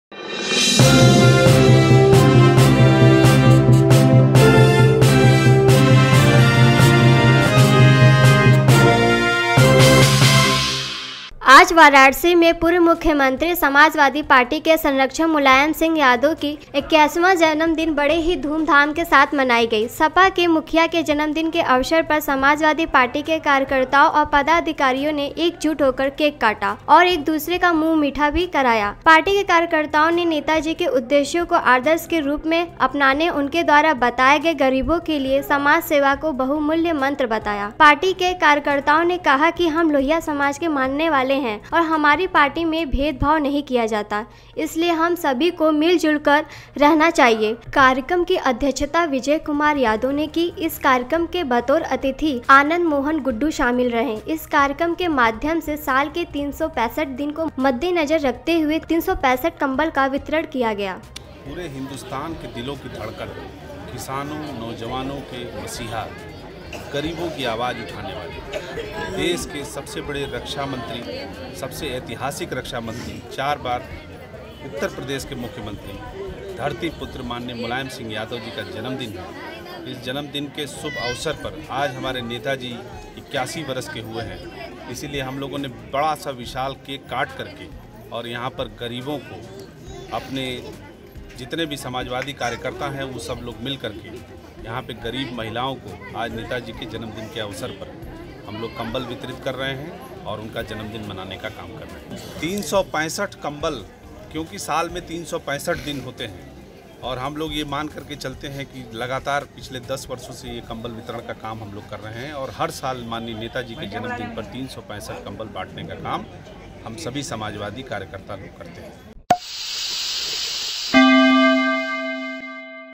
पूर्व CM और SP Party के संरक्षक Mulayam Singh Yadav के Birthday पर बांटे गए कंबल | News Bucket
वाराणसी में पूर्व मुख्यमंत्री और समाजवादी पार्टी के संरक्षक मुलायम सिंह यादव की 81वां जन्मदिन बड़े ही धूमधाम के साथ मनाई गई। सपा के मुखिया के जन्मदिन के अवसर पर समाजवादी पार्टी के कार्यकर्ताओं व पदाधिकारियों ने एकजुट होकर केक काटा और एक दूसरे का मुंह मीठा कराया।

Blankets distributed on the birthday of former CM and SP party mentor Mulayam Singh Yadav - Watch the complete Video For More Details!

Gadgets We Use:

Green Chroma